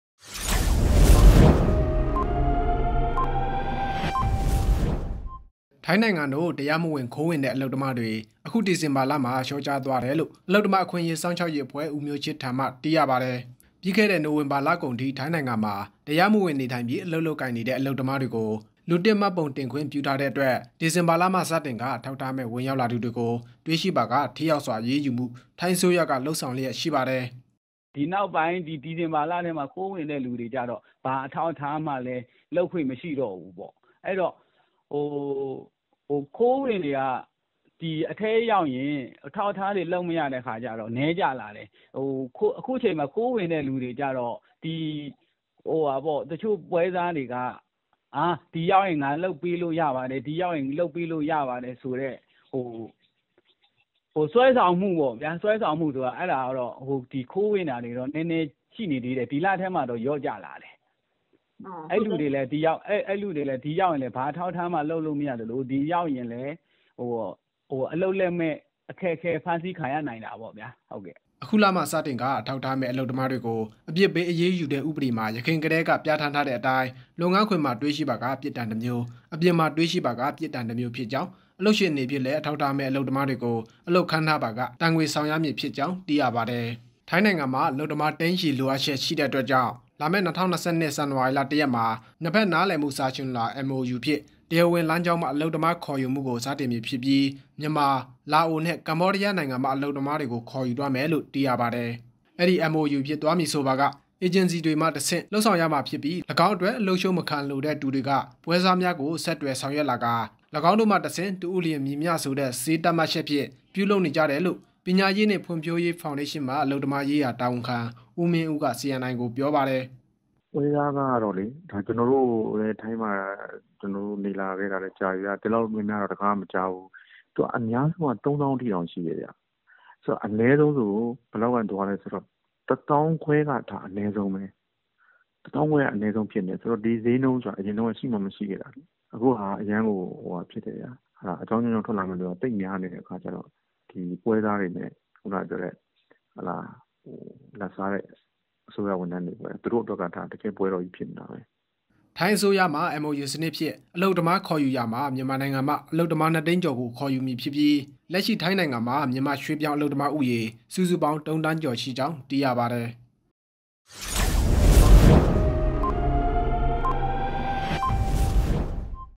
ထိုင်းနိုင်ငံသို့ တရားမဝင်ခိုးဝင်မှု ဒီဇင်ဘာတွင် လျော့ကျလာ
CNI News

၂၀၂၁ ခုနှစ်၊ ဒီဇင်ဘာ ၉

“ဒီ နောက်ပိုင်း ဒီဇင်ဘာလထဲမှာကျတော့ ခိုးဝင်တဲ့သူတွေ ဘာအထောက်အထားမှလည်း လုပ်ခွင့်မရှိတော့ဘူး ပေါ့။ အဲတော့ ခိုးဝင်တွေက ဒီ အထဲရောက်ရင် အထောက်အထားတွေ လုပ်မရတဲ့အခါကြတော့ နည်းကျလာ တယ်။ အခုချိန်မှာ ခိုးဝင်တဲ့သူတွေက တချို့ပွဲစားတွေက ဒီရောက်ရင် ငါ လုပ်ပေးလို့ရပါတယ်။ ဒီ ရောက်ရင် ငါလုပ်ပေးလို့ ရပါတယ်ဆိုတဲ့ ဆွဲဆောင်မှုဆိုတော့ နည်းနည်းရှိနေသေးတယ်။ ဒီလထဲမှာတော့ လျော့ကျလာ တယ်။ အဲလူတွေလည်း ဒီရောက်ရင် ဘာ အထောက်အထားမှ လုပ်မရသလို ဒီရောက်ရင်လည်း အလုပ်လက်မဲ့ အခက်အခဲဖမ်းဆီးခံရနိုင်တာပေါ့ဗျာ” ဟု ဦးမျိုးချစ်က CNI သတင်းဌာန သို့ပြောသည်။

ယခုလမှစတင်ကာ အထောက်အထားမဲ့ အလုပ်သမားများအား အပြစ်ပေးအရေးယူမည့် ဥပဒေမှာ ယခင်ကတည်း က ပြဌာန်းထားသည့်အတိုင်း လုပ်ငန်းခွင်မှာတွေ့ရှိပါက ပြစ်ဒဏ်တစ်မျိုး၊ အပြင်မှာတွေ့ရှိပါက ပြစ်ဒဏ် တစ်မျိုး ဖြစ်ကြောင်း၊ အလုပ်ရှင်အနေဖြင့်လည်း အထောက်ထားမဲ့အလုပ်သမားများကို အလုပ်ခန့်ထားပါက ဒဏ်ငွေ ဆောင်ရမည် ဖြစ်ကြောင်း သိရသည်။